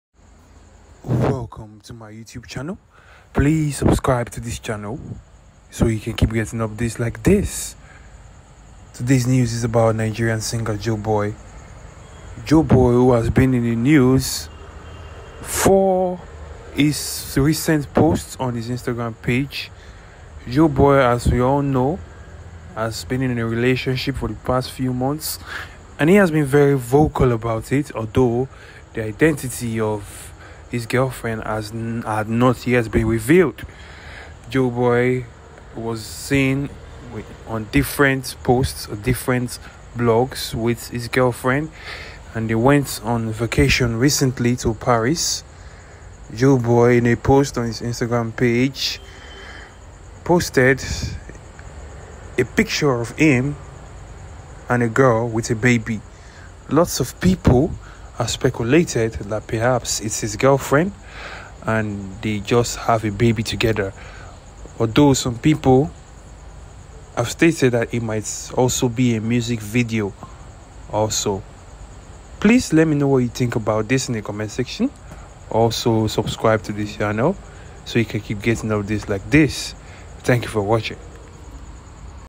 JOEBOY and his girlfriend welcome NEW BORN BABY
Singer Joeboy and his girlfriend welcome new born baby